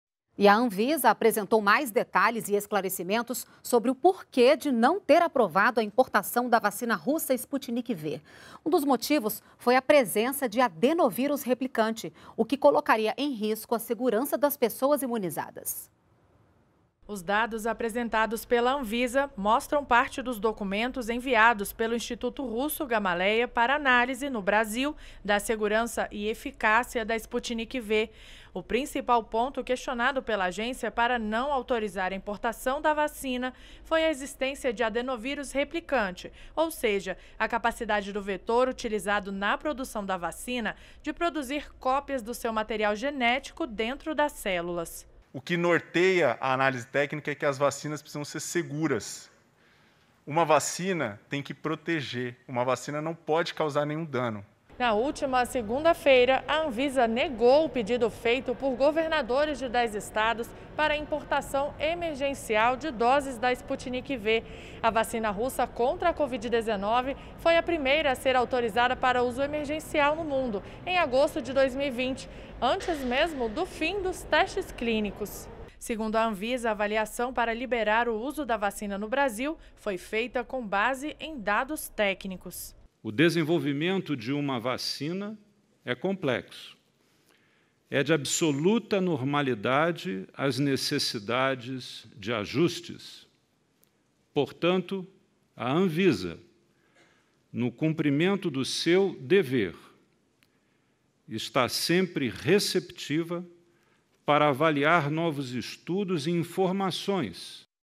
Anvisa reforça existência de adenovírus replicante na Sputnik V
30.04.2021 – A Anvisa apresentou mais detalhes sobre o porquê de não ter aprovado a importação da vacina russa Sputnik V. Um dos motivos foi a existência de adenovírus replicante na vacina, o que colocaria em risco a segurança das pessoas imunizadas.